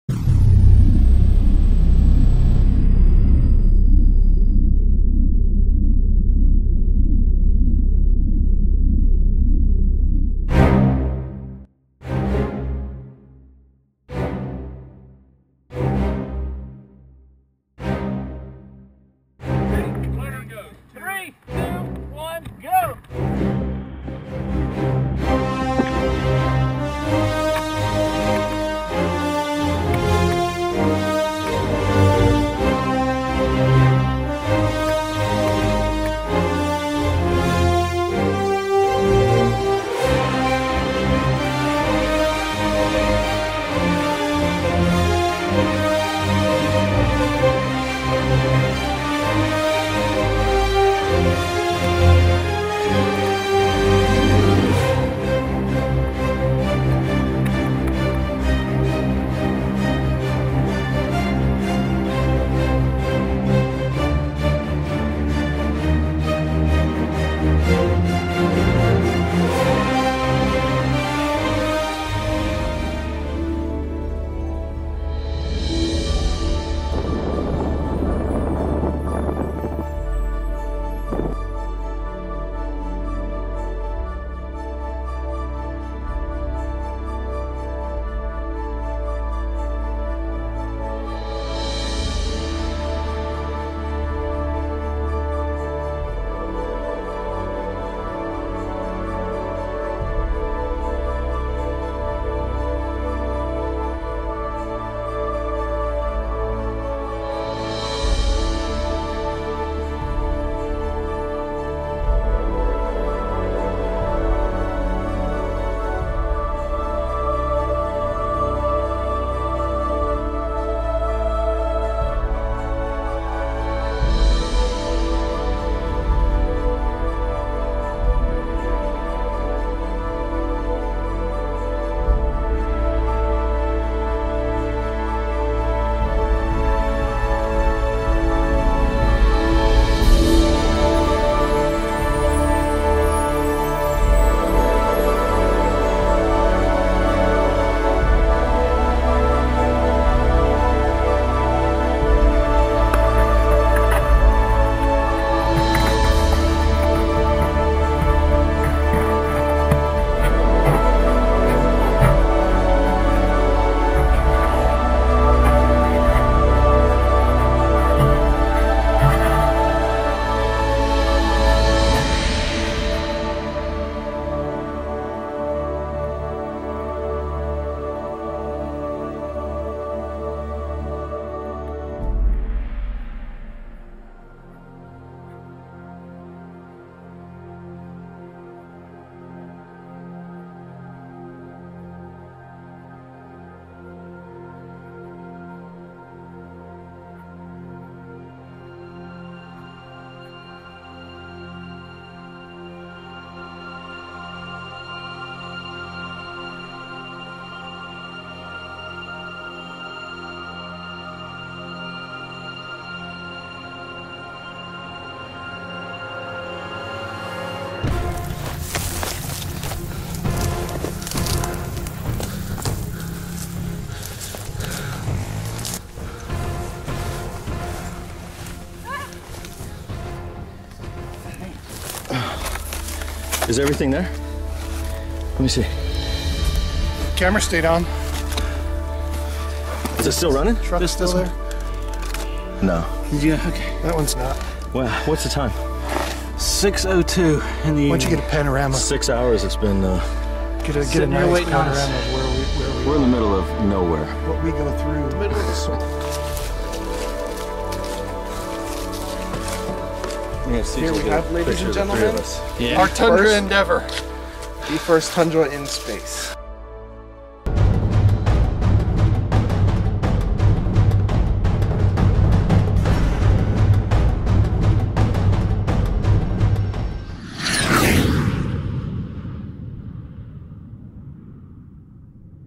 First Toyota Tundra in Space
We were so inspired by the Felix Baumgartner jump, as well as the Tundra pulling the Space Shuttle Endeavour, we decided it would be fun to launch a Toyota Tundra into Space. Check out how we did this seemingly impossible feat. Amazing views from 90k feet and the Scavenger hunt to find the package after it landed 90 miles away from the launch site.